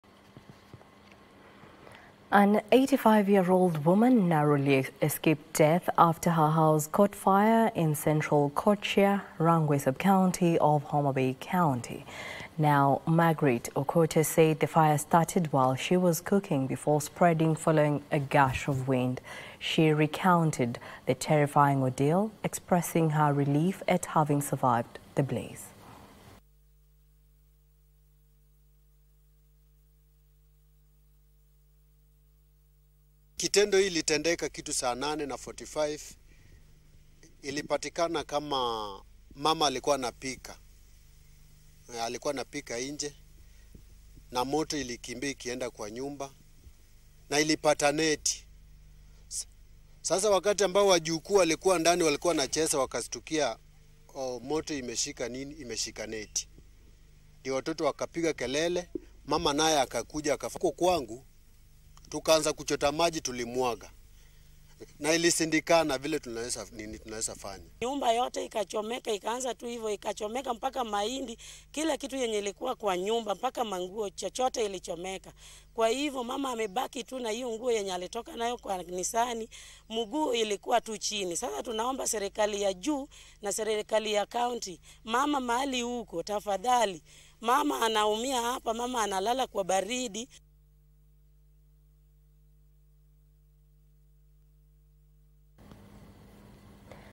85-year-old woman narrowly escapes death after her house caught fire
An 85-year-old woman narrowly escaped death after her house caught fire in Central Kochia, Rangwe sub-county of Homa Bay County.
Margaret Okota said the fire started while she was cooking, before spreading following a gush of wind. She recounted the terrifying ordeal, expressing her relief at having survived the blaze.